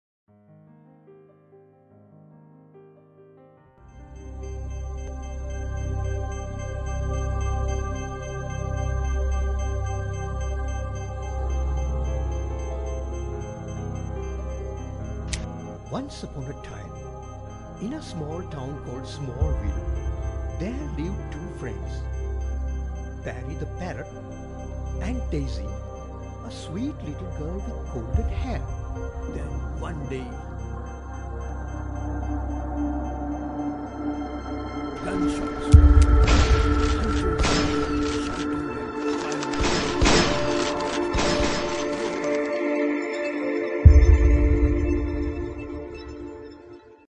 Diploma Film Clips
A short documentary animation film
story conveys the messege for wild life conservation and
 aims to make people aware of ecosystem .